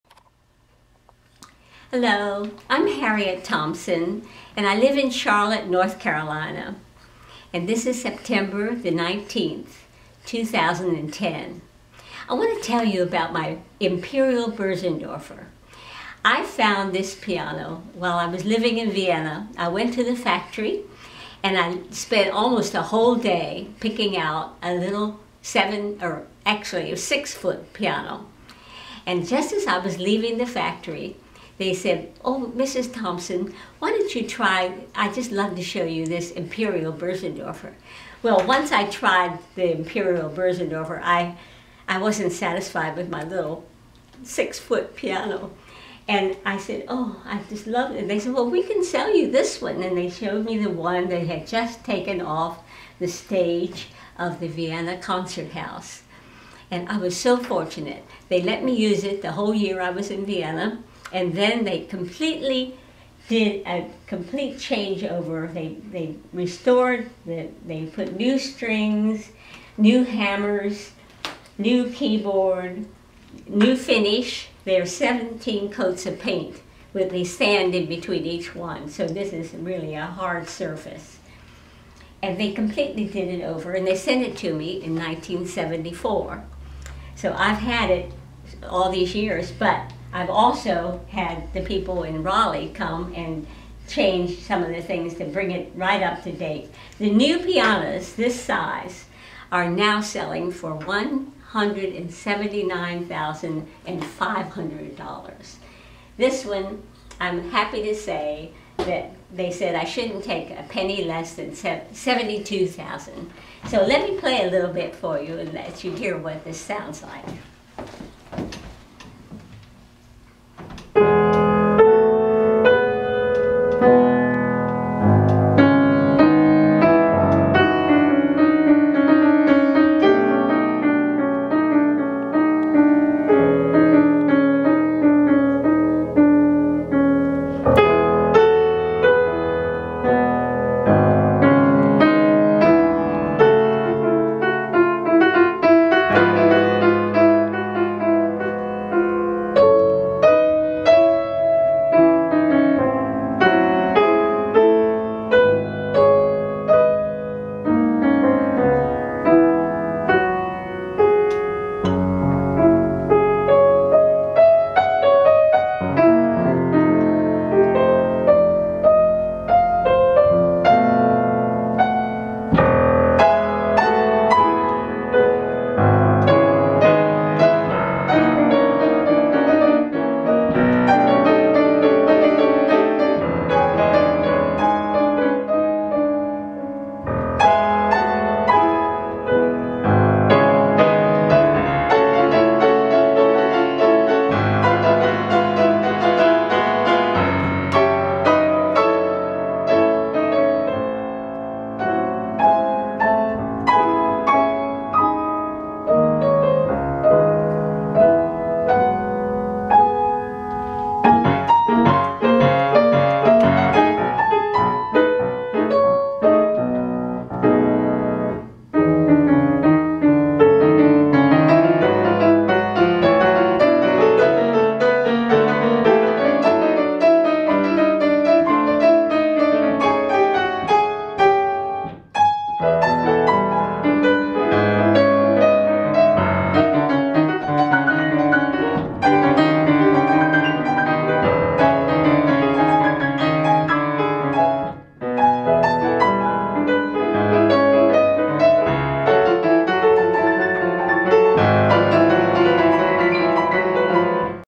Harriette Line Thompson presents her Bosendorfer Imperial Concert Grand Piano
Harriette Line Thompson shares how she acquired her 9'6" Imperial Grand from the Bosendorfer factory in Vienna. This piano is being offered on ebay during the month of September 2010.